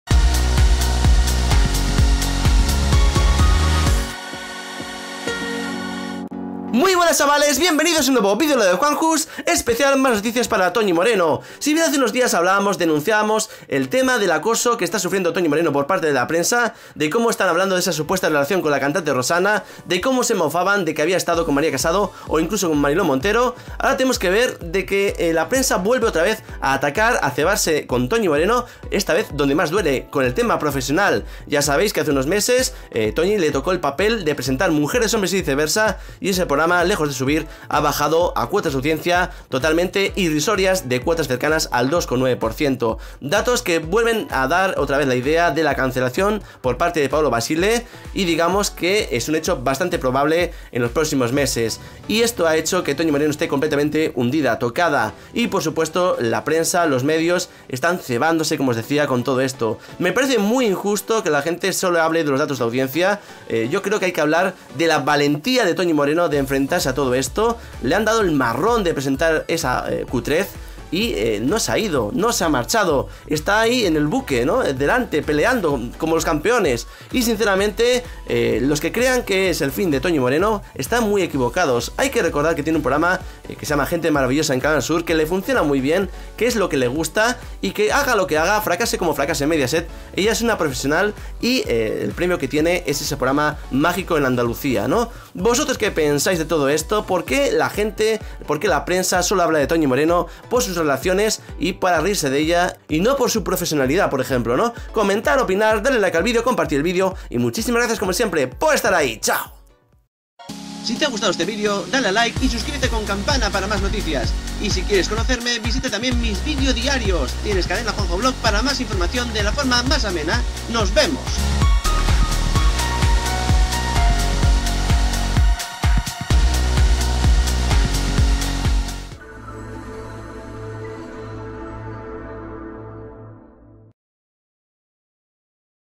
Malas noticias para Toñi Moreno
La antigua presentadora de Viva la Vida y actual de Mujeres y hombres y viceversa se las tiene que ver con la prensa que sugiere que actualmente tiene una relación con la cantante Rosana .
Fotos, testimonios y portadas son el día a día de Toñi moreno .
Por otro lado suenan malas noticias en mediaset / telecinco por los malos datos tanto del programa de Emma como del suyo con fuertes rumores de cancelación de ambos.